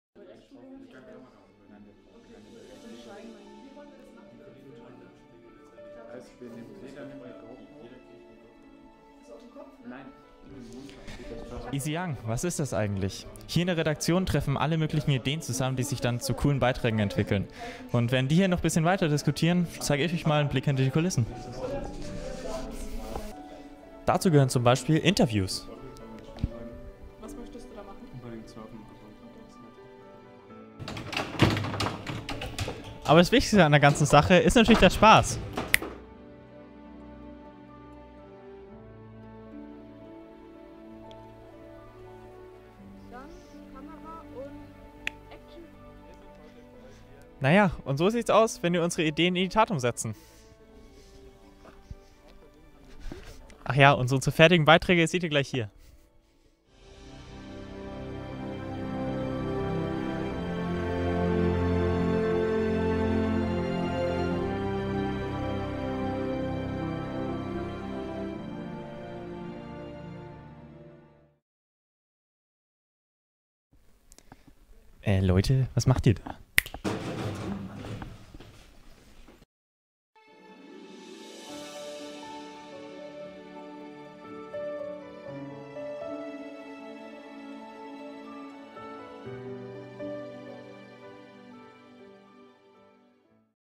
WAS IST EASYOUNG TV? | Imagevideo
Was ist easYoung TV eigentlich?
Wer steckt dahinter?
Was machen wir überhaupt?

Wir geben euch mal einen kleinen Einblick in unsere Jugendredaktion und zeigen euch wer wir sind und was wir machen.

Ihr habt jetzt Lust bekommen auch bei uns mitzumachen?
Kein Problem! Schau einfach in einer unserer Redaktionssitzungen immer Montags um 18 Uhr im Jugendmedienzentrum Connect in Fürth vorbei!